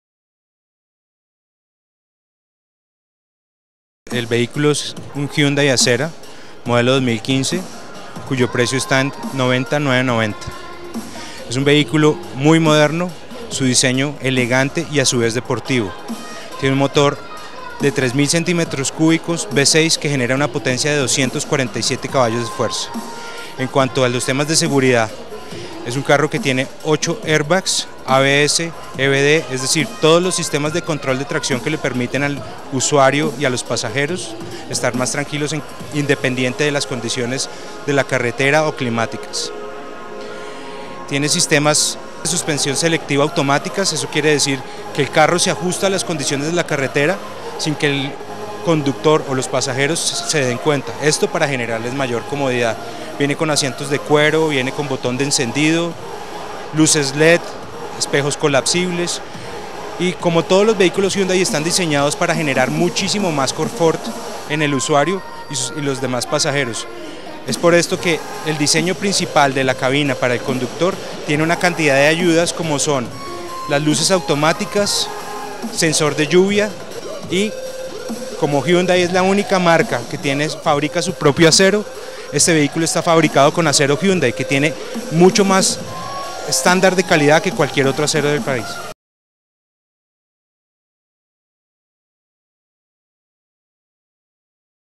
Conozca el nuevo Hyundai Azera de venta en Colombia
Hyundai lanza en Colombia la versión renovada del Hyundai Azera, con su motor V6 de 3 litros y 247 caballos de fuerza a 6,400rpm,  uno de los más potentes de su segmento. Este carro de lujo está exhibido en el Salón del Automóvil de Bogotá.